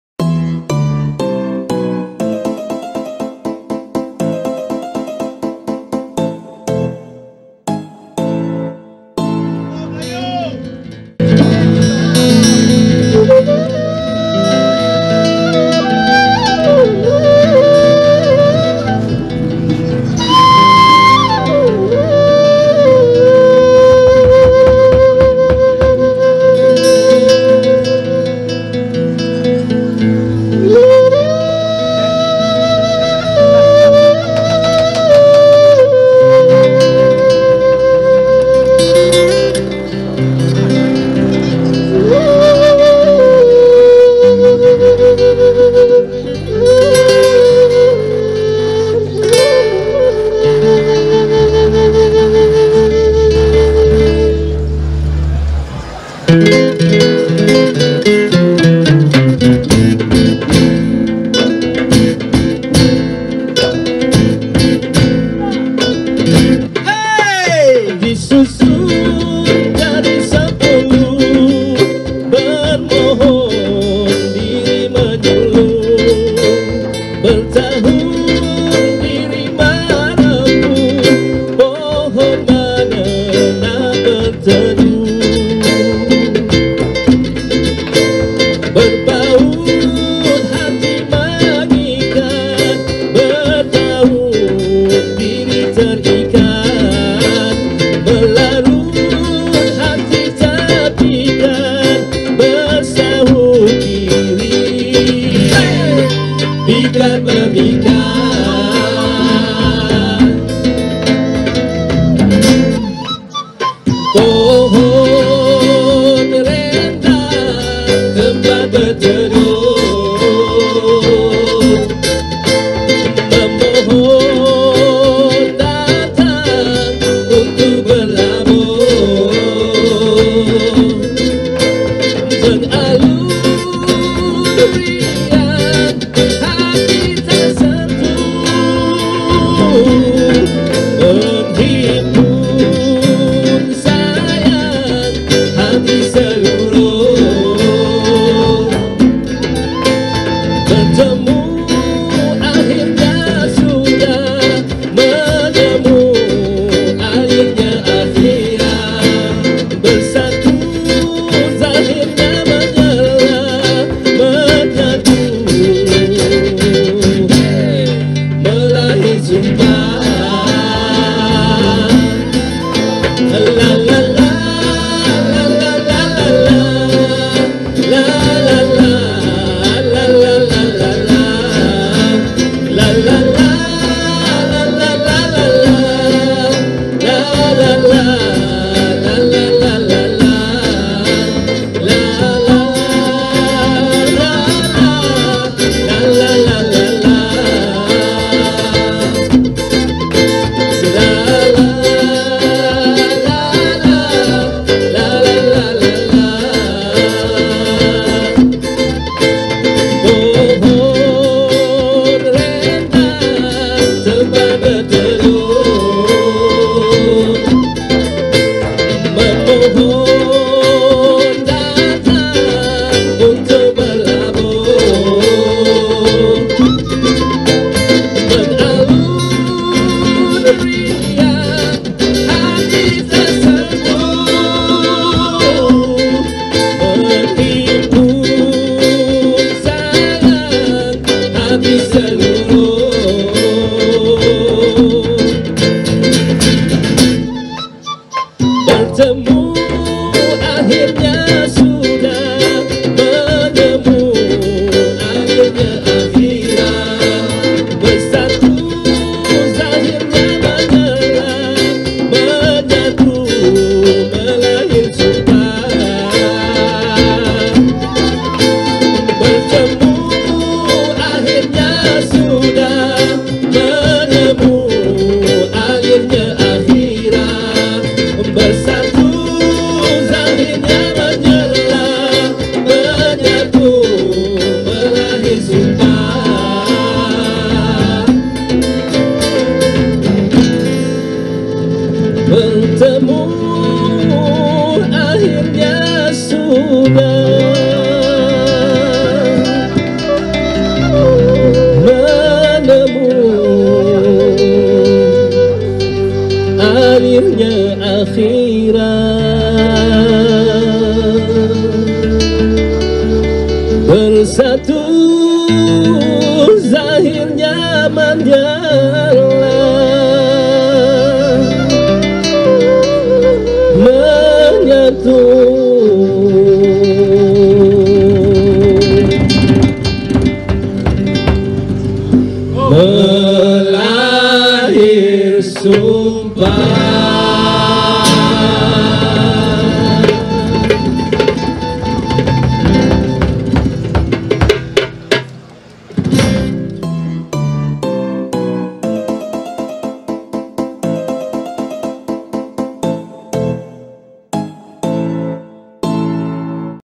Melahir Sumpah - Dendang Anak - Tok Cindai
Sambutan majlis melayu raya idilfitri 1443H pada 28 hb mei 2022
anjuran KMIB MALAYSIA
bertempat di Balai Adat Gelanggang Tok Cindai

Di sambut dengan penuh meriah.
Di hadiri dengan 73 Ngo Dan ribuan Pengunjung penuh Adat dan Berbusana Melayu.

Pintu Istana Jangan Pecah
Baju Raja Jangan Dicarik